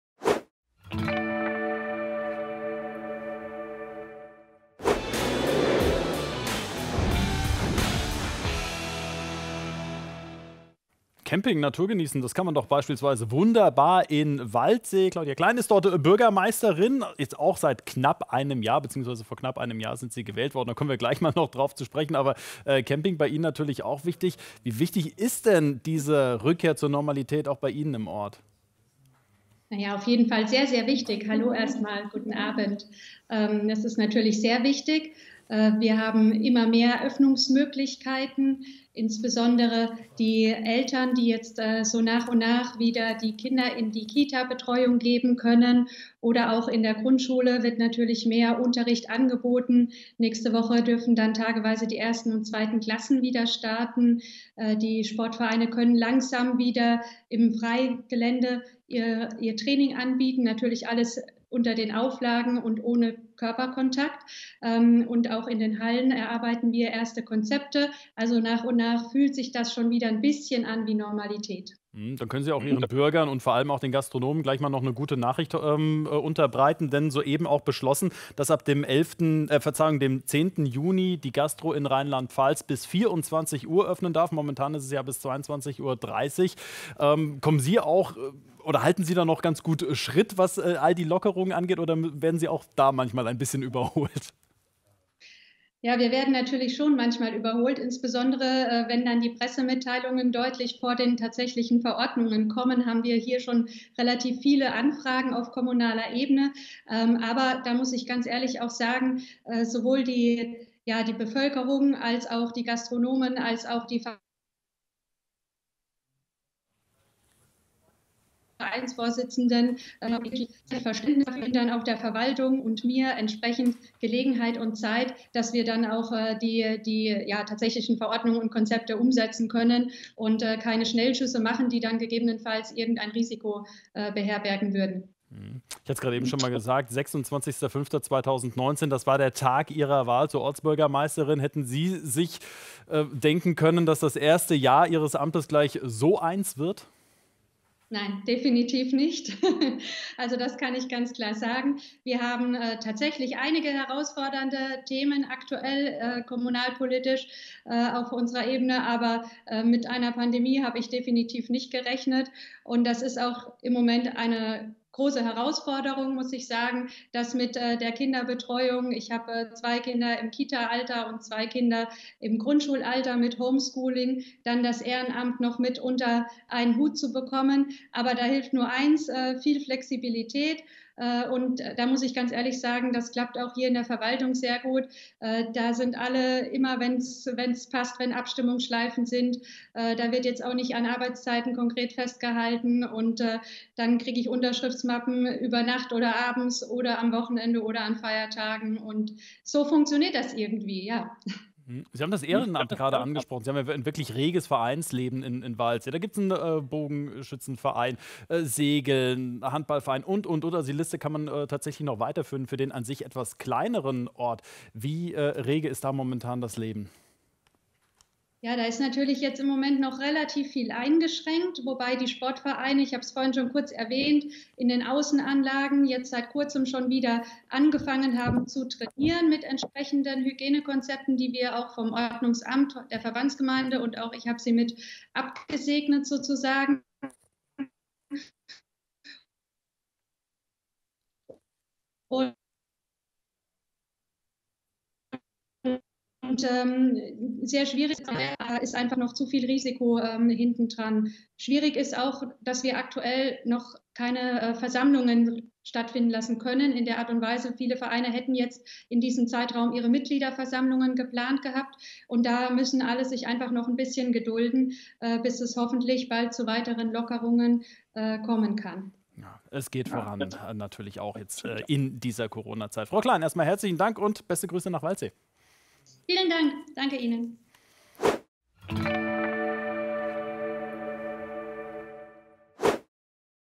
Ortsbürgermeisterin von Waldsee im Gespräch | RON TV
In unserer Serie sprechen wir mit Bürgermeistern über die Herausforderungen durch die Corona-Krise und die Auswirkungen auf ihre Gemeinde. Heute schildert Claudia Klein die Situation in Waldsee.